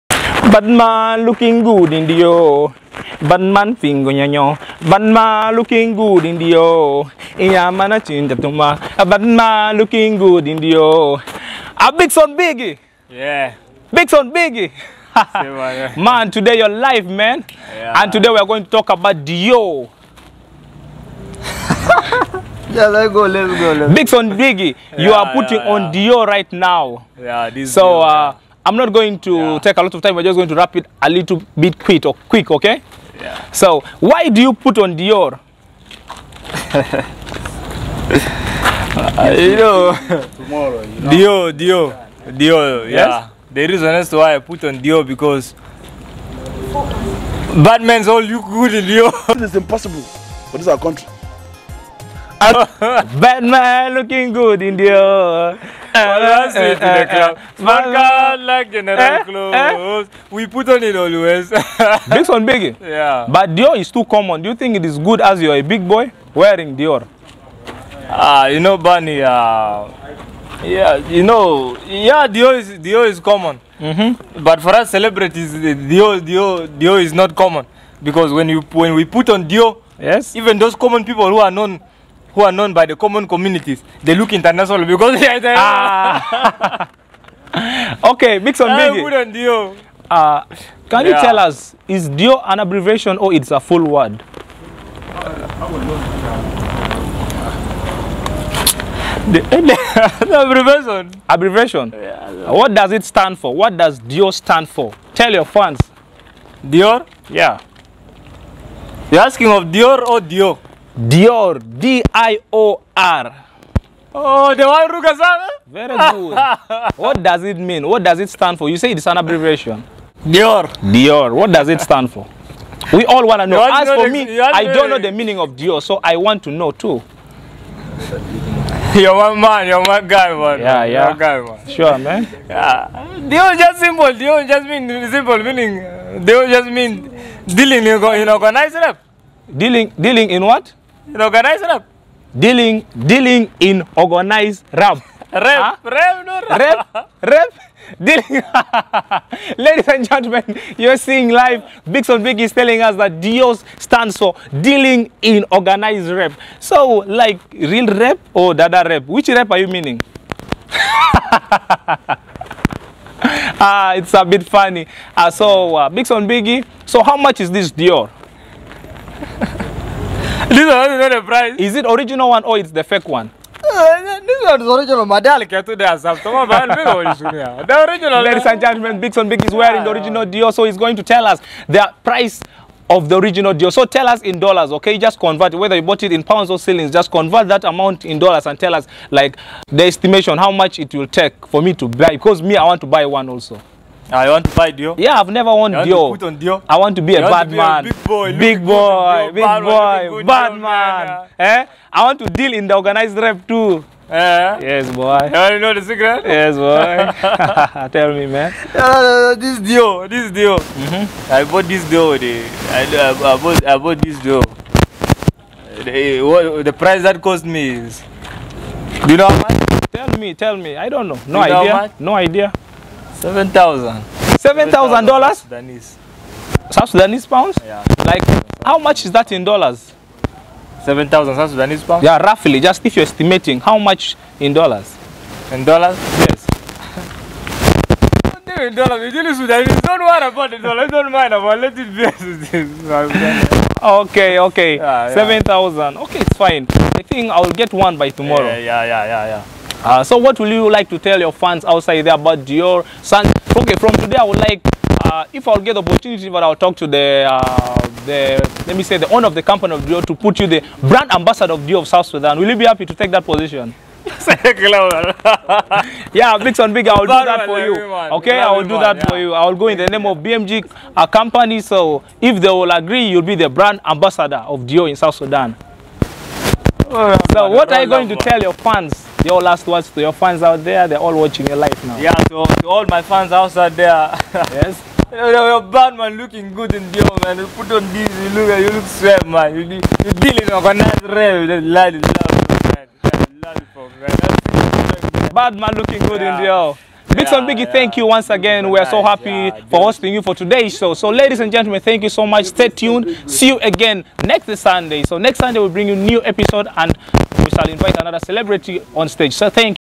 Meaning Of Dior : Ruger Dior vs Bmg Comedy
Thats the true meaning of Dior explain by bigson biggy